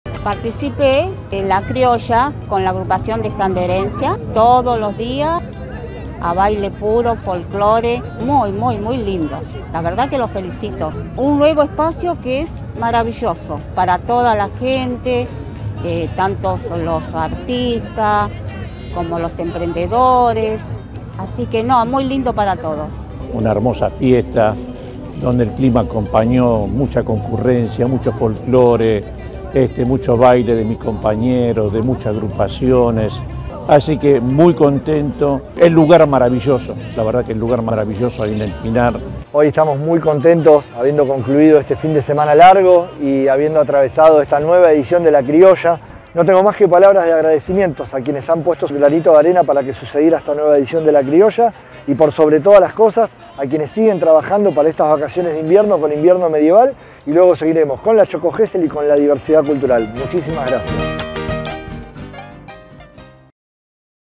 Una multitud disfrutó de la Fiesta Tradicional “La Criolla” 🇦🇷
👉🏽 Durante todo el fin de semana, el Anfiteatro del Pinar “Tito Allo” fue el escenario de este evento, que año a año atrae a residentes y turistas para disfrutar de la mejor gastronomía típica en el escenario natural del Bosque geselino.

Artesanos, emprendedores, artistas, y mucho mas en cuatro dias a pura cultura y tradicion ❤️🌲